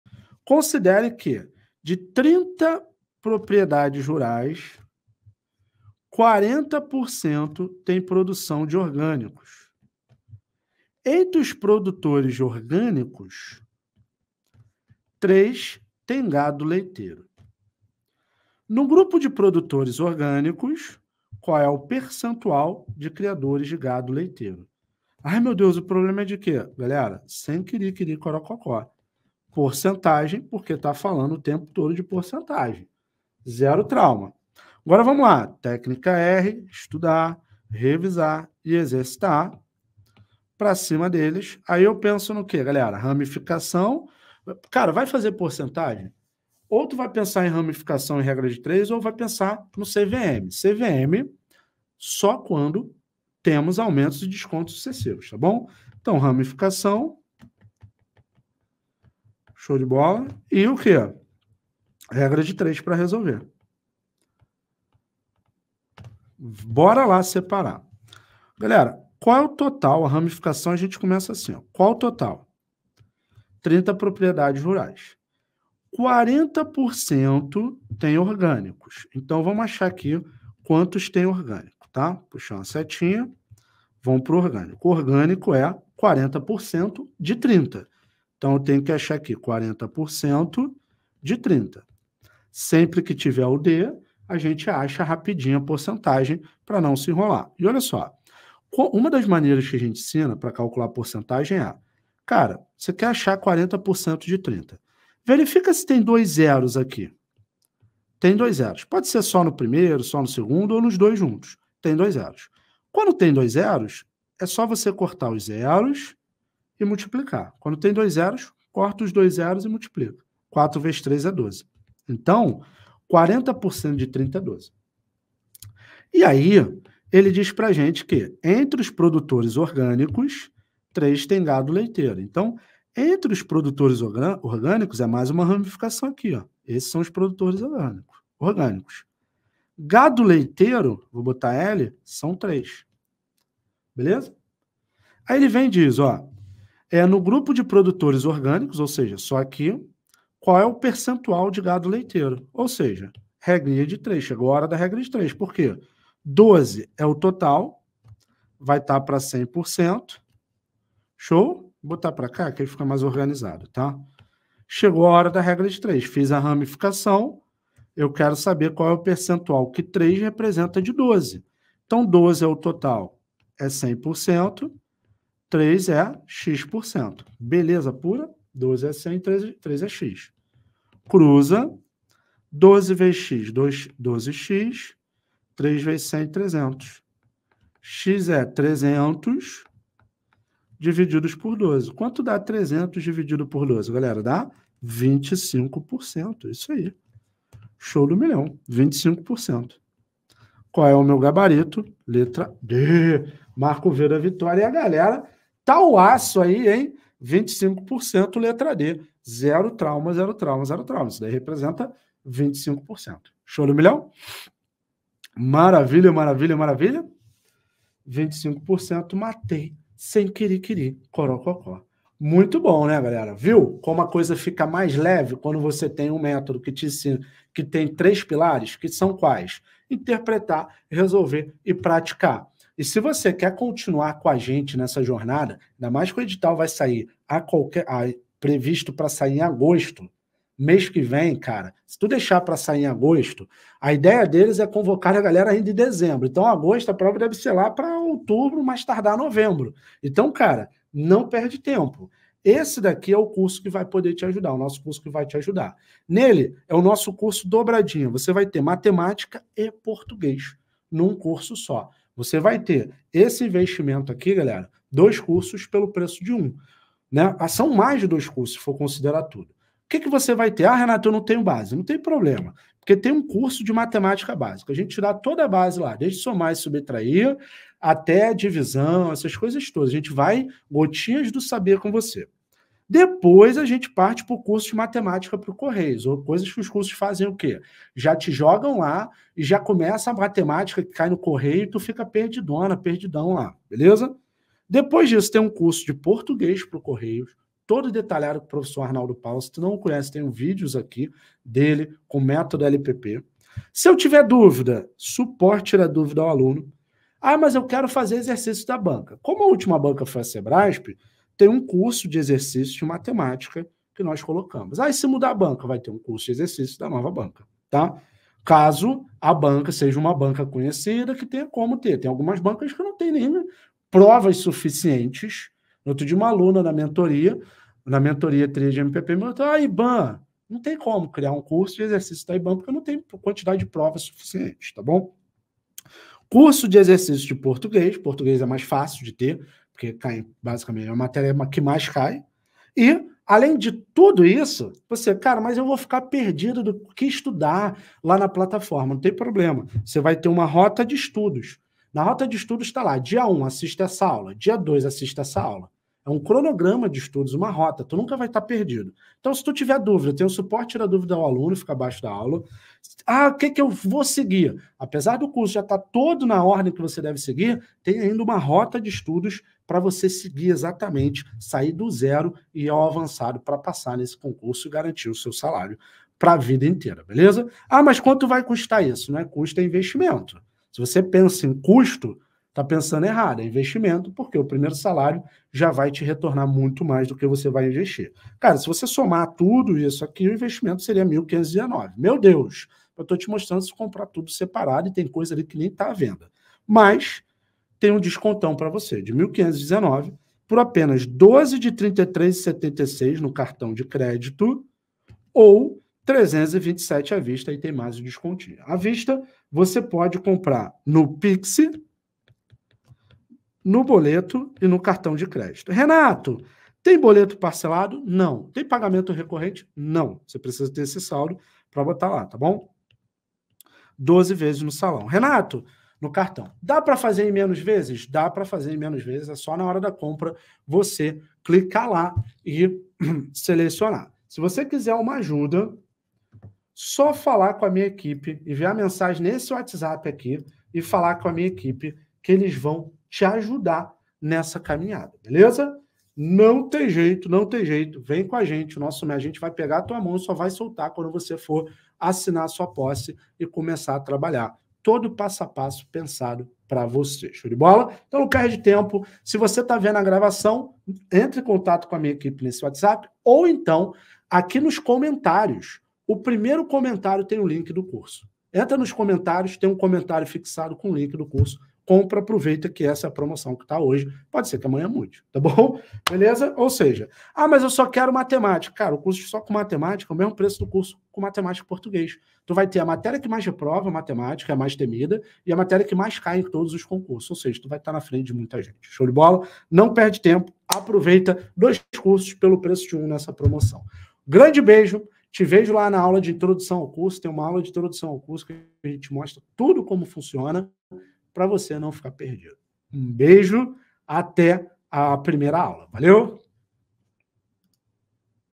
Matemática para Correios | Porcentagem agora ficou fácil
Você precisa assistir esse vídeo onde nosso professor, Renato Oliveira, mostra Matemática para Concurso dos Correios 2024 com Método MPP.

Você terá um passo a passo para aprender a matéria mais temida por todos e acertar mais de 80% ou mais nas questões de Matemática para o concurso dos Correios 2024.

Agora basta dar o play no vídeo e aprender Matemática para Concurso dos Correios 2024 com Método MPP